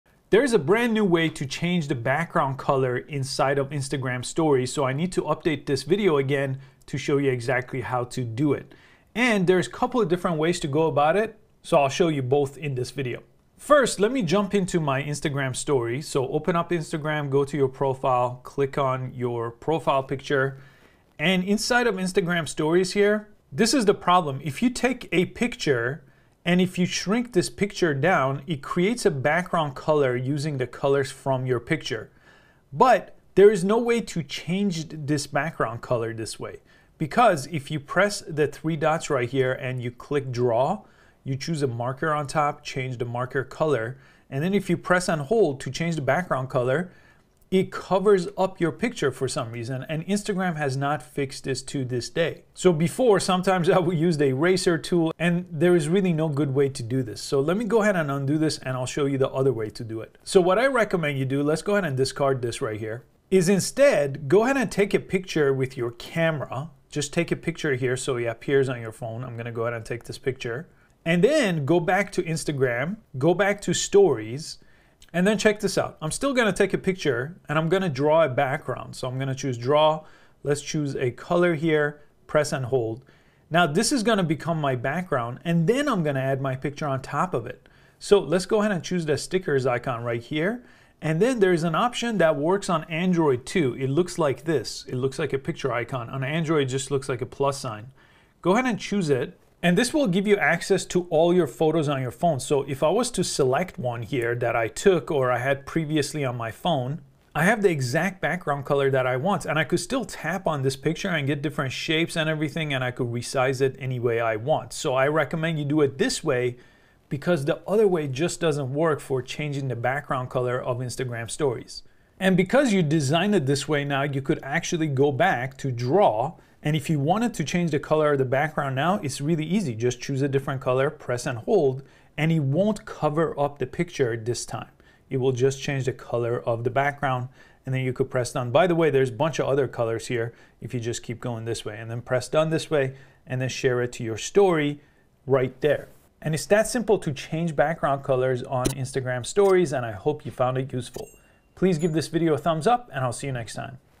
How to Change Background Color on Instagram Stories - New Update
There is a new way to change the background color on Instagram Stories.
You can do this to create a solid color background and add images and text on top of it.

If you open Instagram Stories and take a picture, you won’t be able to change the background color.  But there is a workaround.  You can take a picture with your phone, then open up Instagram Stories.  You can then create a solid color background using the draw tool after you take a picture.

Then, you can use the sticker option and bring in the picture that you previously took outside the Instagram app.  If you tap the picture, you’ll get several options for styling it.